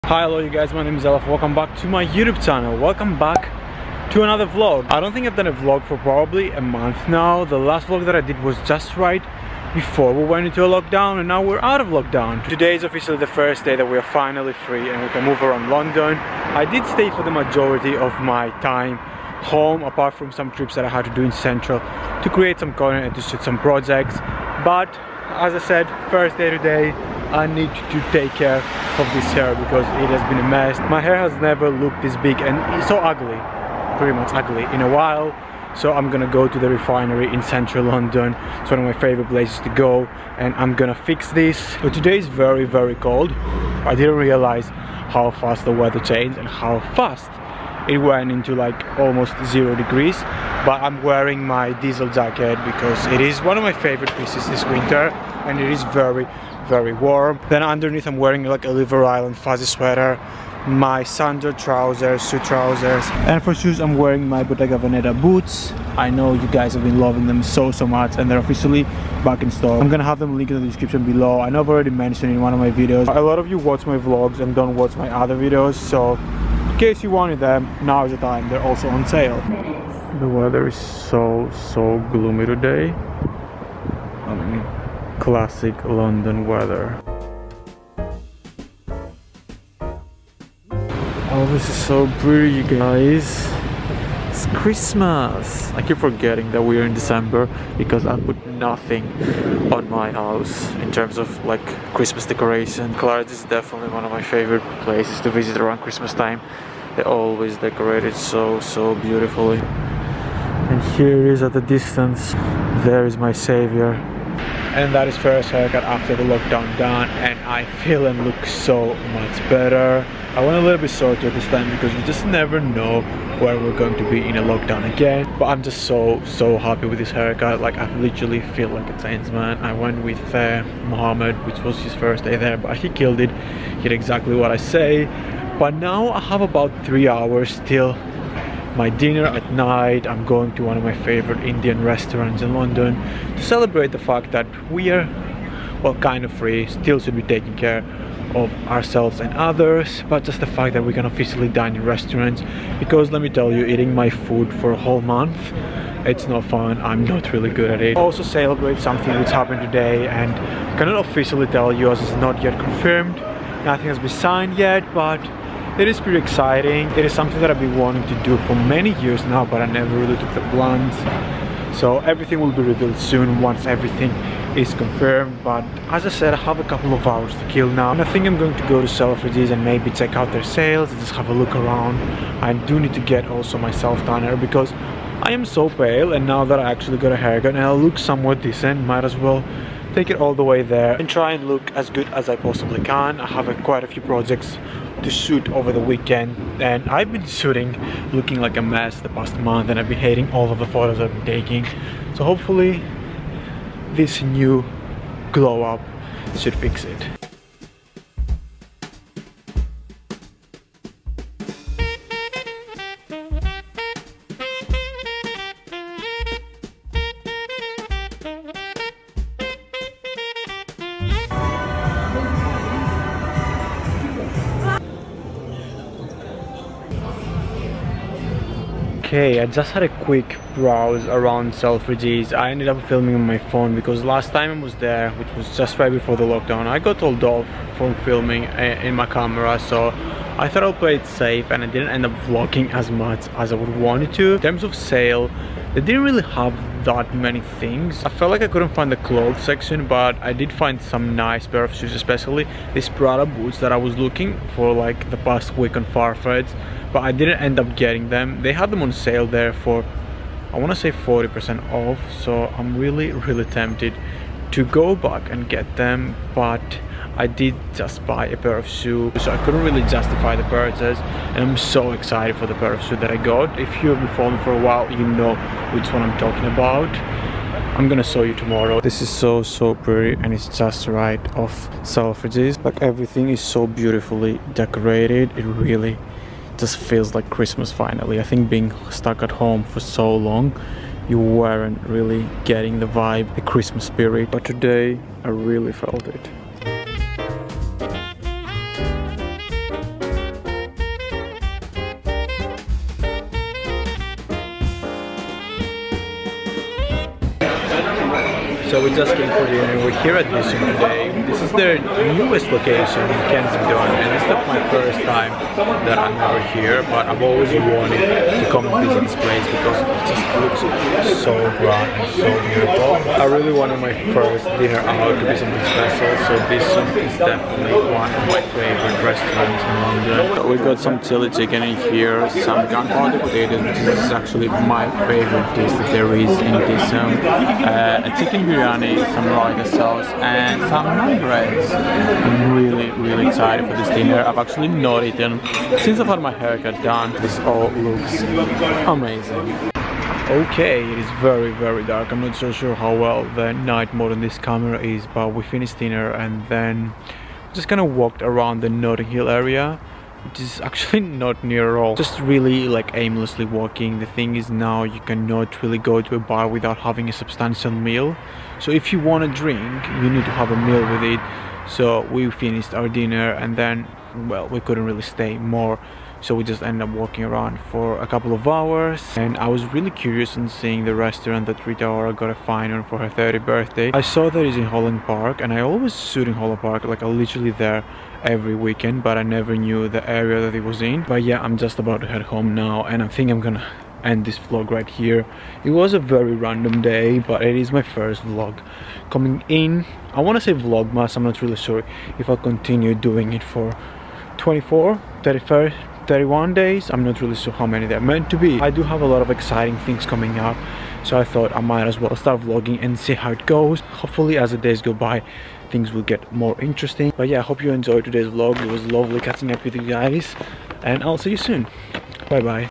First Day after Lockdown 2.0 | Vlogmas Day 2
Hi guys and welcome back to my Youtube channel, in today's video I am going to central London in the first time in a month after the Lockdown in the UK has been lifted.

Items Mentioned;
Bottega Veneta Dupes;
Bershka Chunky Boot;

My Favorite Oversized Trousers/Wide Fit Trousers;
- Oversized Tapered Blue Trousers;
- Navy Wide Leg Seersucker Trousers;
- High Waisted Pinstripe Trousers;
- Wide Leg Trousers in Check;
-Oversized Tapered in Grey Flannel;
- Asos Vintage Jeans;
- Relaxed Cargo Trousers;